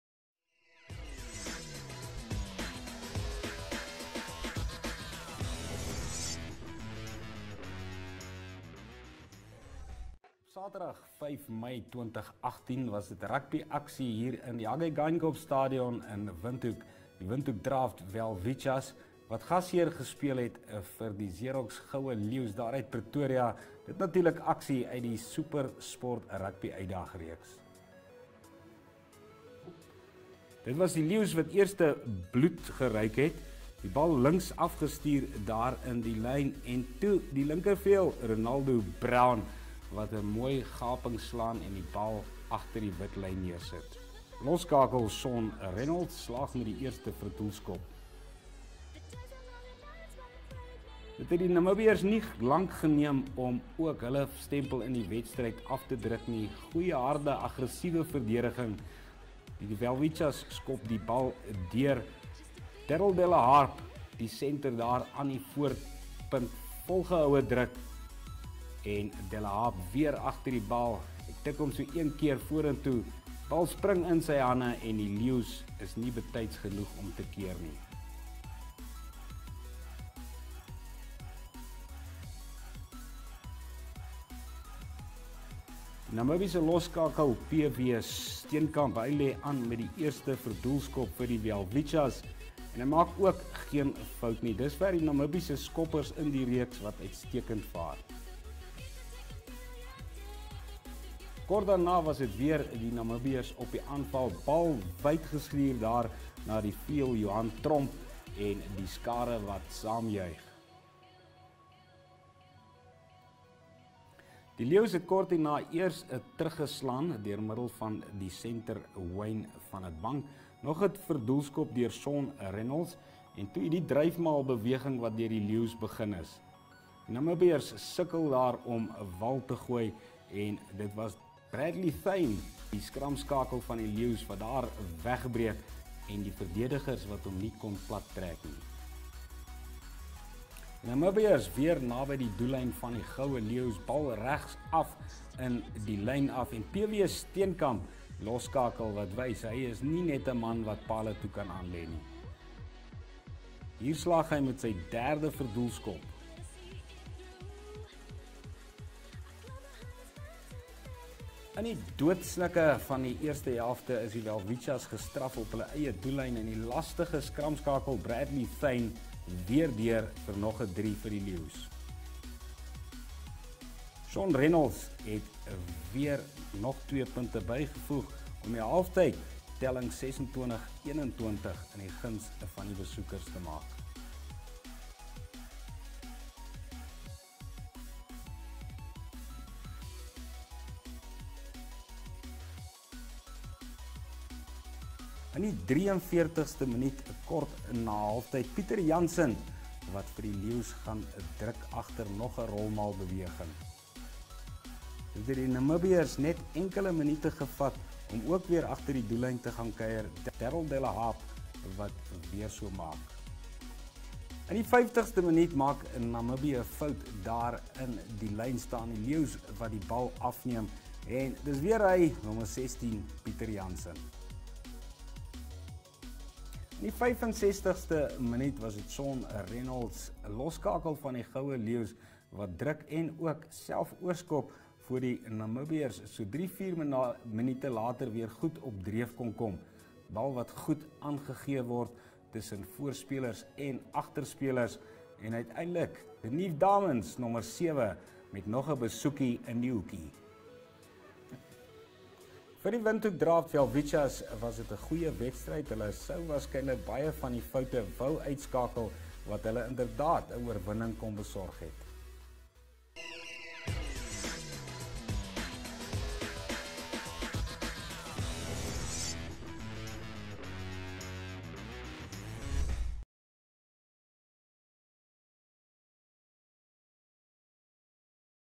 Rugby highlights - Windhoek Draught Welwitschias vs Xerox Golden Lions
Saturday 5 May 2018, Hage Geingob Stadium, Windhoek
SuperSport Rugby Challenge
Windhoek Draught Welwitschias (21) 35 - Xerox Golden Lions (26) 45